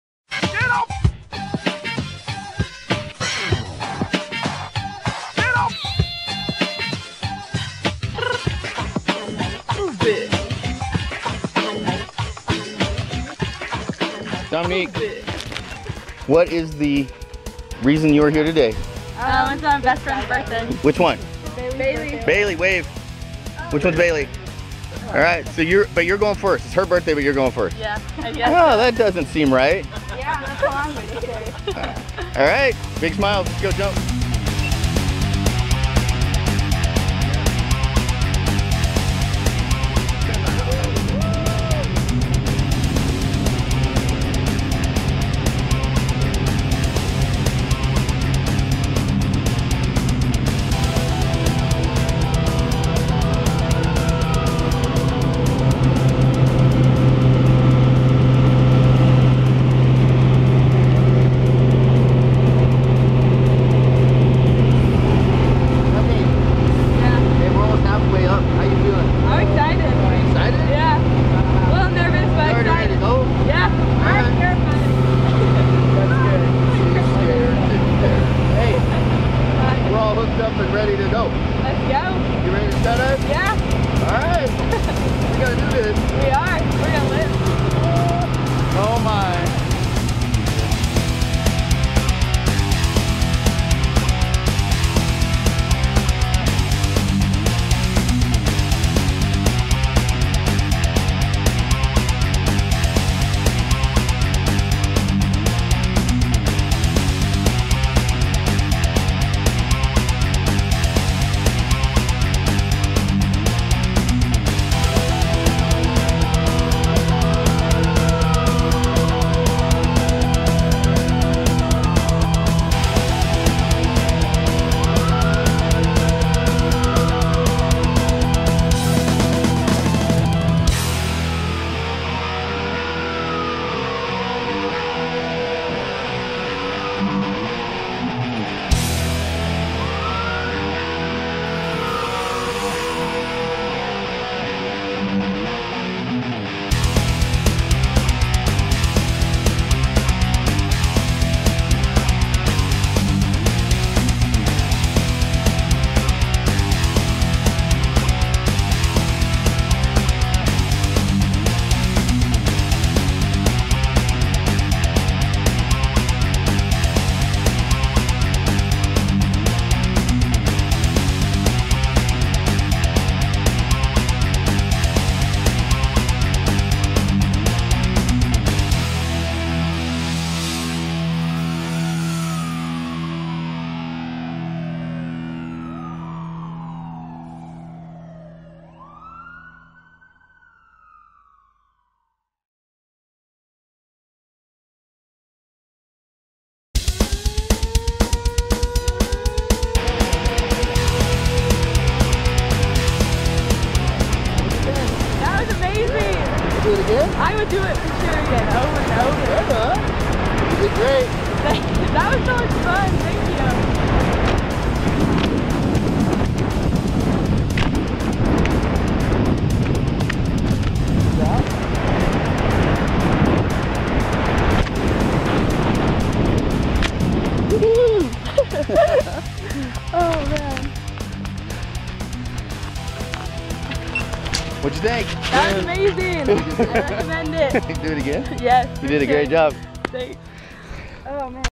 Dominiqe Ramirez's DZONE Tandem skydive at Skydive Idaho!
Dominiqe Ramirez's DZONE Tandem skydive at Skydive Idaho on Sunday, June 26, 2016!